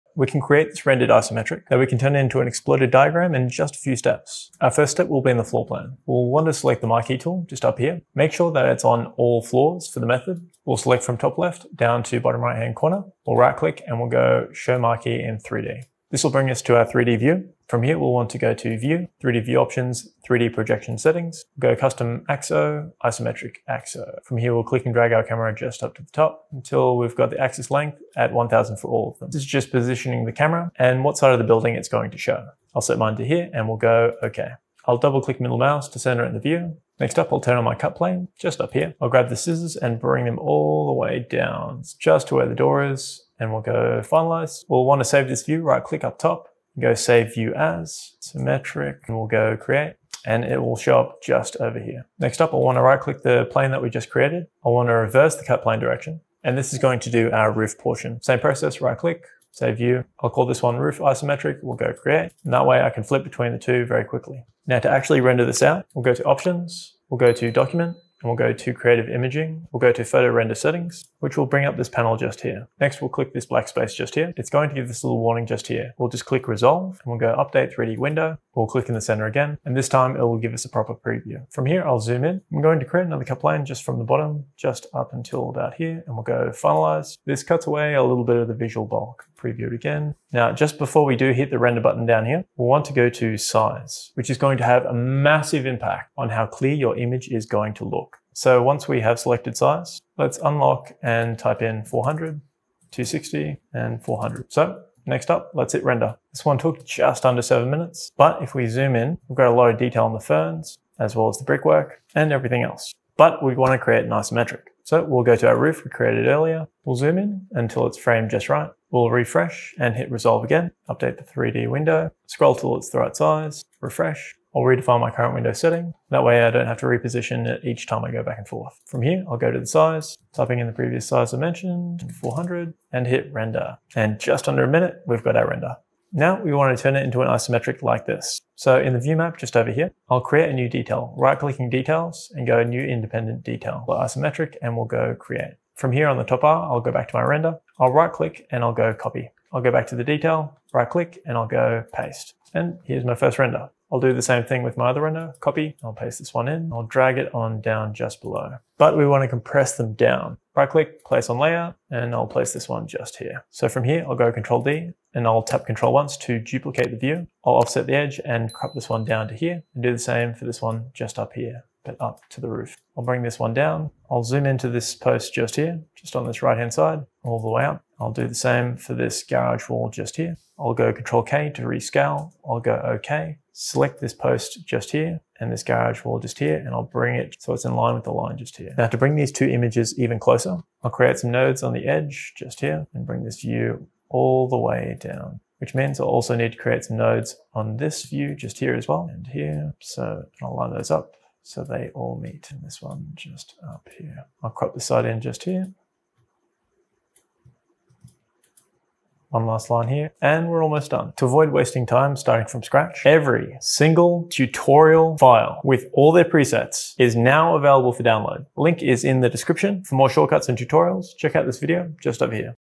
ArchiCAD Tutorial: Rendered Isometric... in 3 minutes
How we can create isometric renders in just a few minutes, all inside of Archicad.

✂️ Cut down your learning curve, 👇access ALL the ArchiCAD tutorial files

00:00:00 Start
00:00:22 Projection Settings
00:00:39 Setting Cut Plane
00:00:53 Roof isometric
00:01:10 Render Settings
00:01:30 Render Window settings
00:02:10 Setting up the exploded isometric
00:03:30 Accessing the files